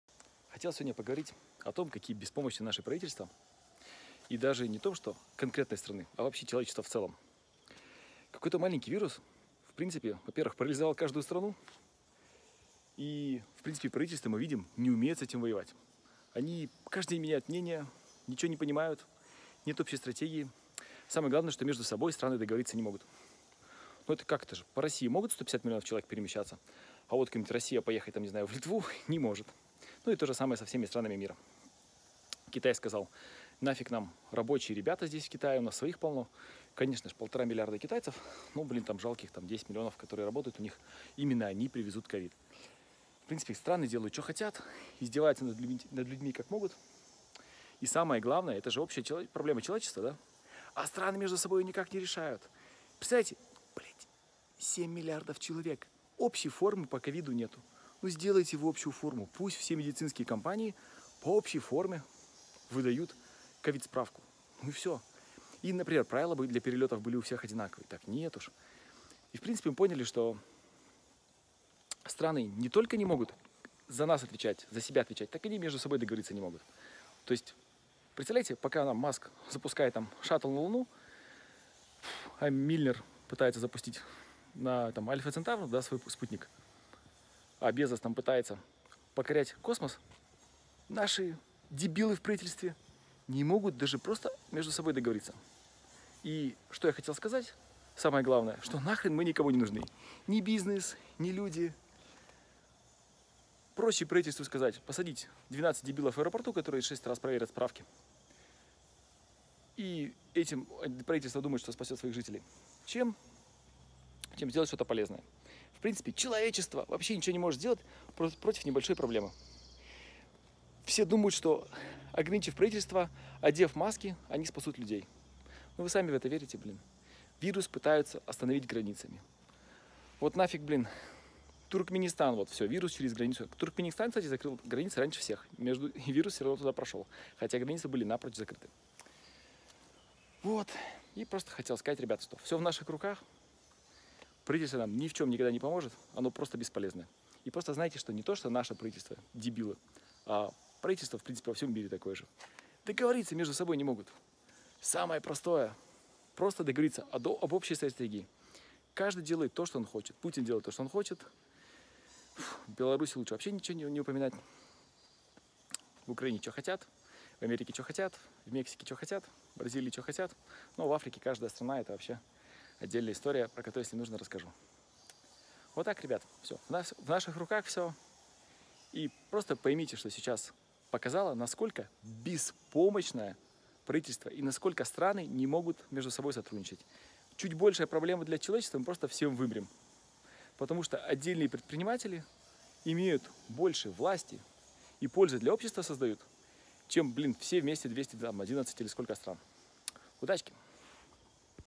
Ковид — страны беспомощны (part 1)
В периоды мировой нестабильности мы надеемся, что правительства стран смогут справиться со страхом и решить глобальную проблему пандемии. Однако, похоже, Филипп Борцов смотрит на ситуацию под другим ракурсом: каждая страна в отдельности бессильна, и совместное преодоление проблемы кажется слишком тяжёлой задачей… Есть ли надежда?